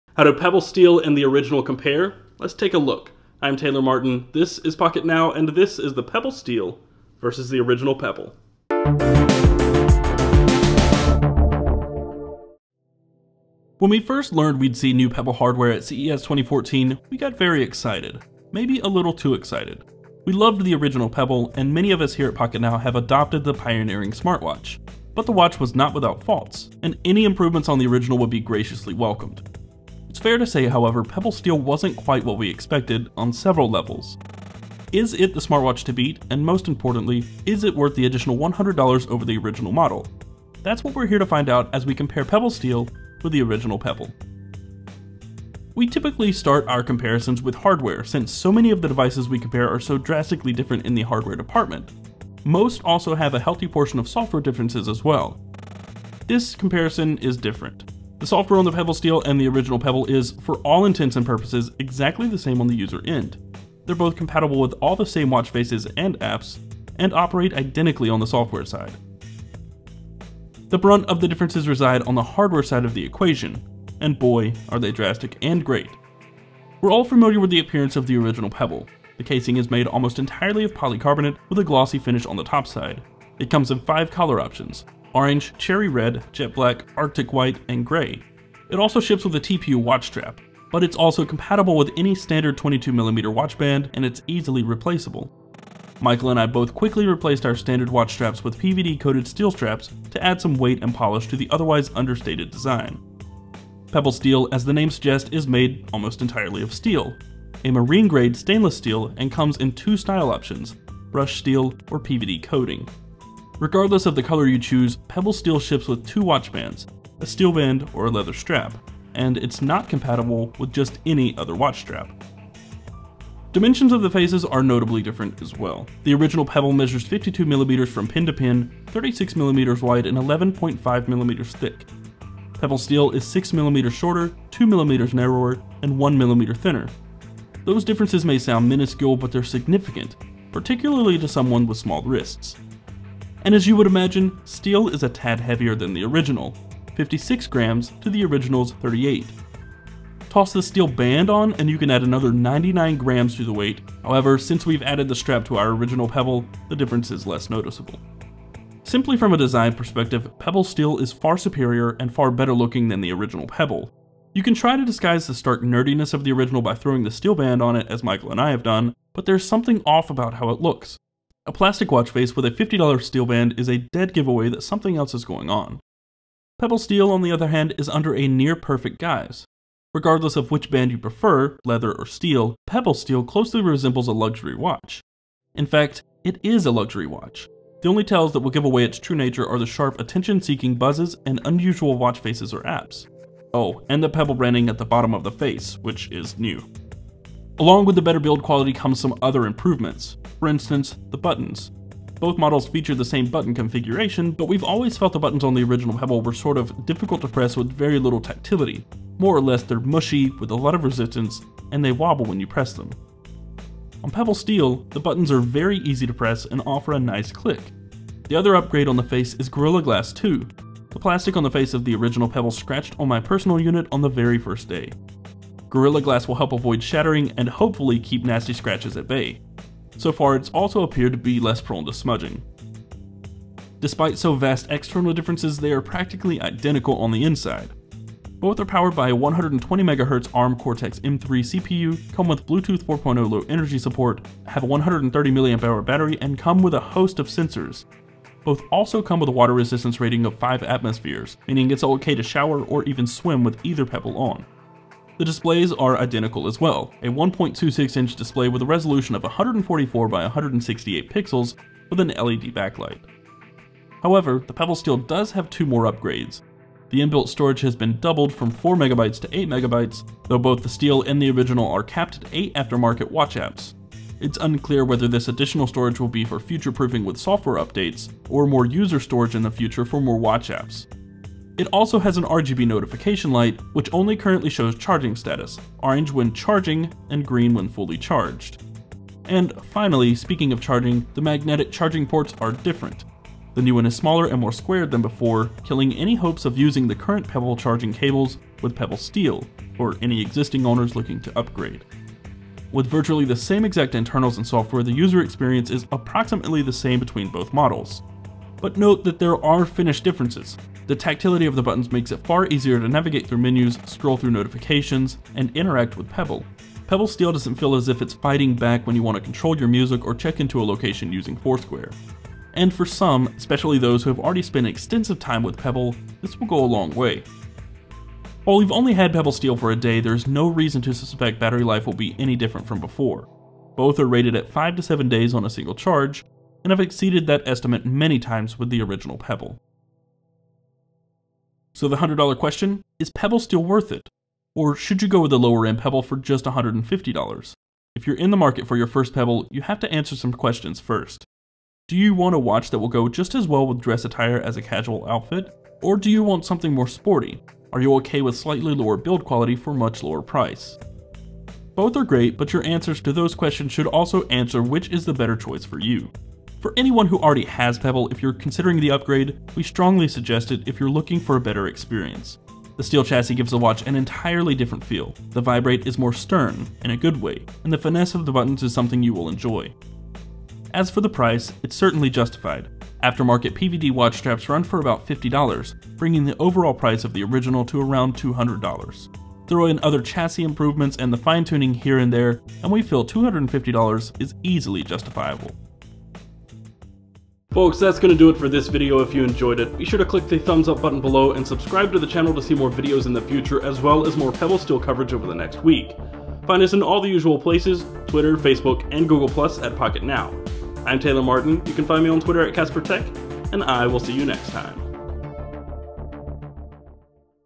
Pebble Steel vs Pebble | Pocketnow
How does Pebble's higher-end smartwatch, Pebble Steel, compare with the original? Is the new watch worth the higher price? And what are the benefits of Pebble Steel over the old model? Those are the questions we set out to answer in this Pebble Steel vs Pebble comparison!